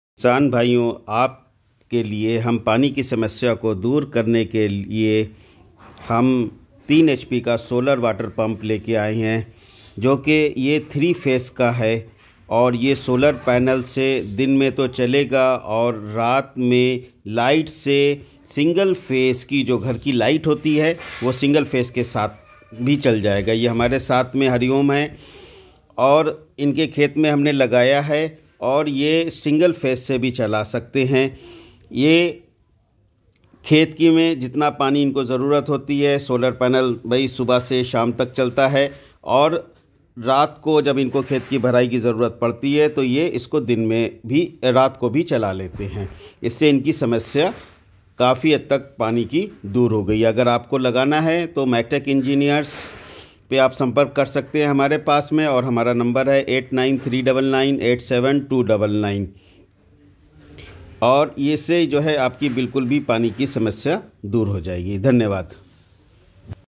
Maktech Engineers 8939987299,9899941483
3 Hp solar water pump 3 phase moter running by 1 phase and solar panels 9 number
Maktech Engineers Ghanta ghar above bank of maharashtra
Shahjahanpur
242001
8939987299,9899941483